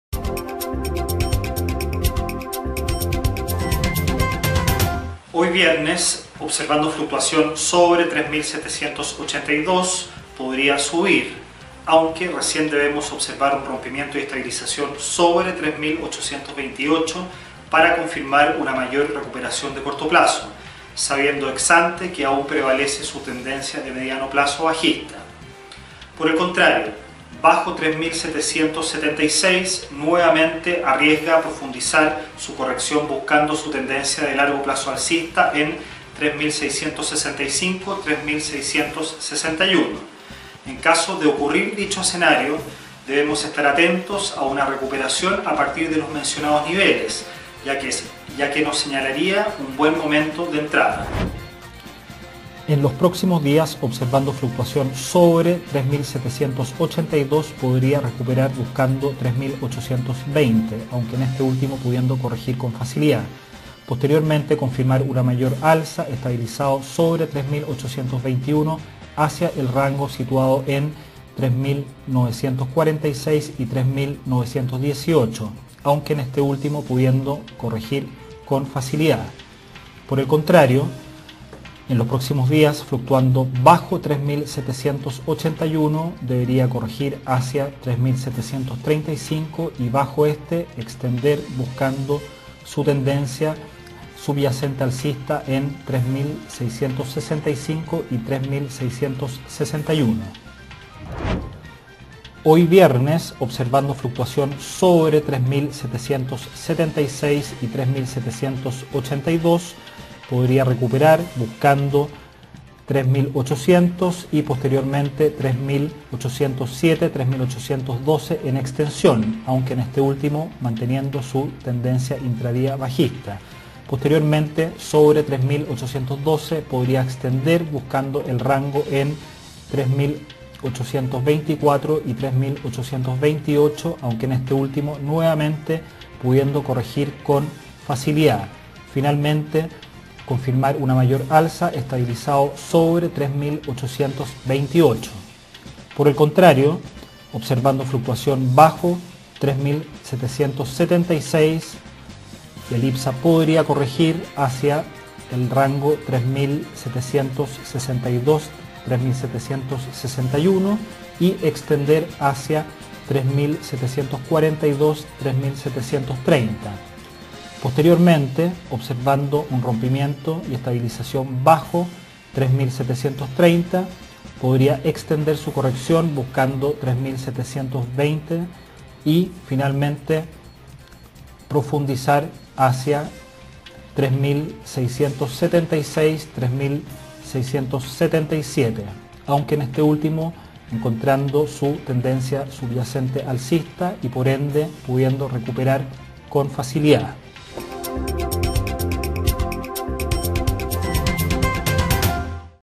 Análisis BetaStock: Observando IPSA fluctuar bajo 3776, arriesga corregir hacia 3676-3666
Observando IPSA fluctuar bajo 3776, arriesga corregir hacia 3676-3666